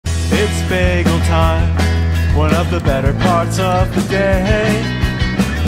It's Bagel Time at Brooklyn Bridge Bagel Works in Downtown Chico, California 🥯 │ Travel Chico
The perfect place to start your day: Brooklyn Bridge Bagel Works 🥯😊

Enjoy freshly baked bagels and experience that New York City flavor from Brooklyn Bridge Bagel Works! Enjoy dozens of bagel flavors and schmears (including vegan options!), and arrive early to snag two bags of their day-old bagels for only $1!

🥯 Brooklyn Bridge Bagel Works
📍 117 W 2nd St Unit A, Chico
⏰ Daily 7am-1:30pm